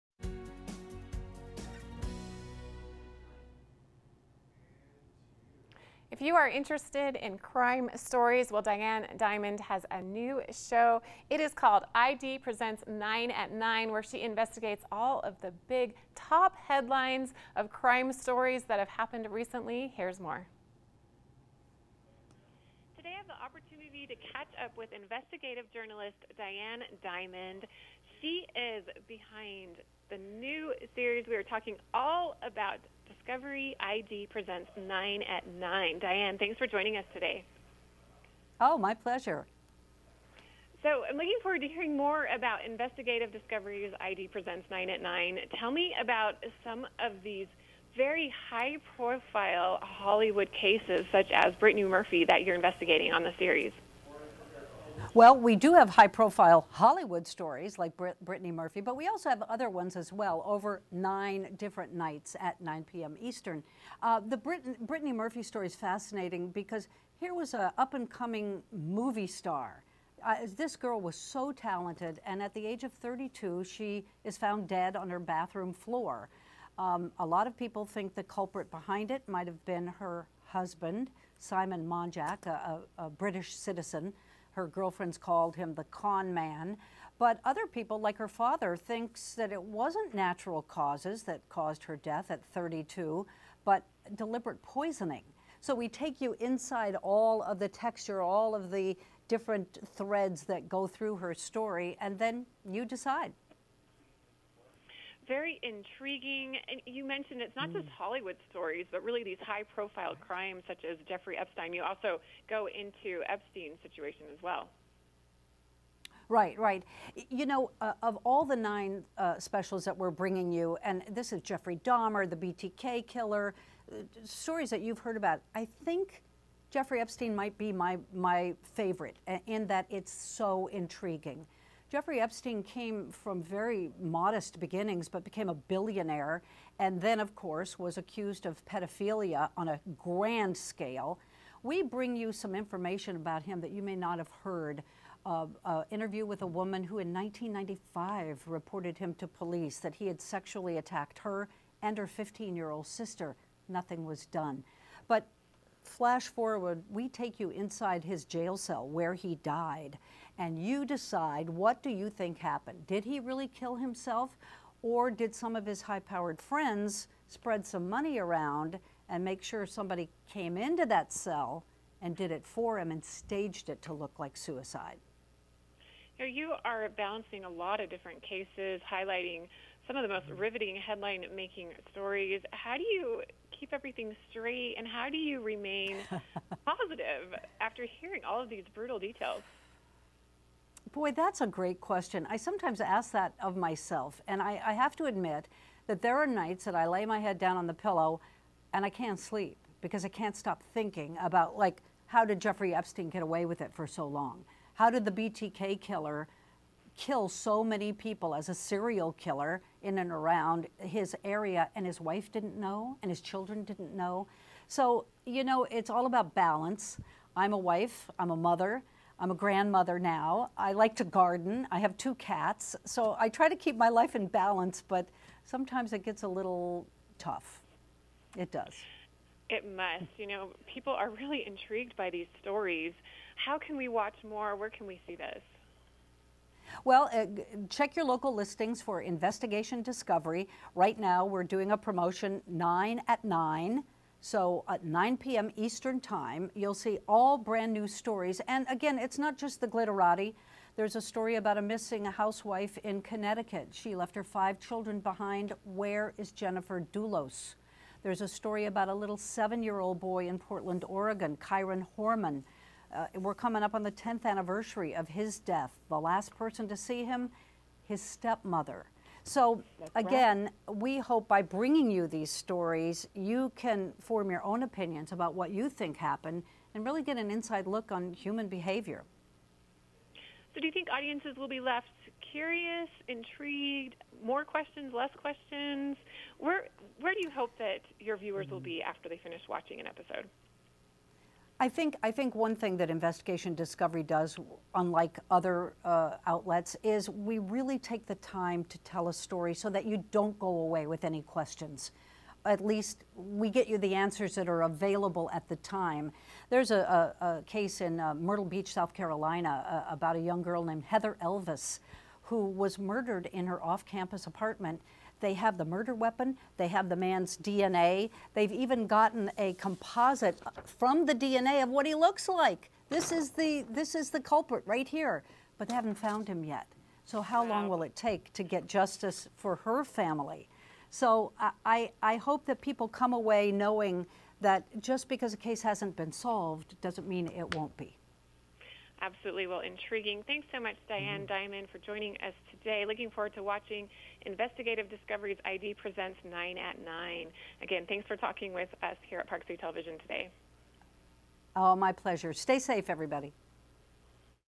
Investigative Discovery’s ID Presents: Nine at 9
Investigative reporter Diane Dimond goes deep behind the headline-making stories in Investigative Discovery’s ID Presents: Nine at 9. Learn more in this interview on PCTV and then watch this show and get your true-crime fix as Diane Dimond highlights six of the most riveting headline-making stories.